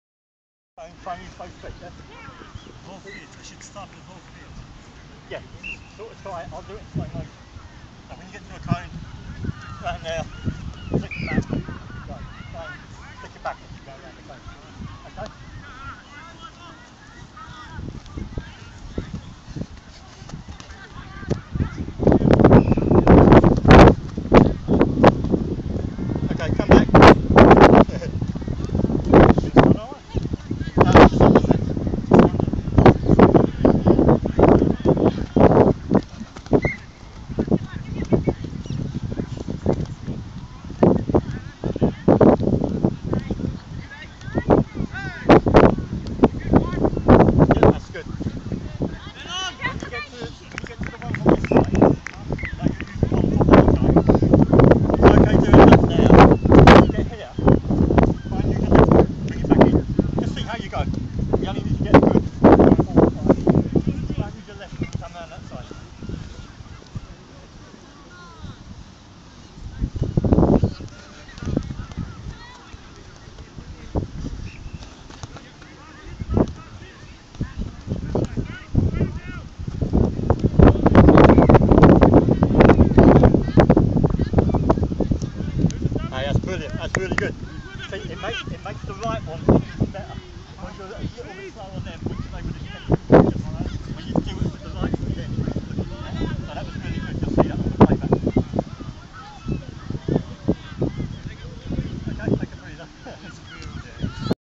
Nikolce Stojanovski ball skills with UEFA PRO License coach Ian Gannon III
Nikolce Stojanovski ball skills with UEFA PRO License coach Ian Gannon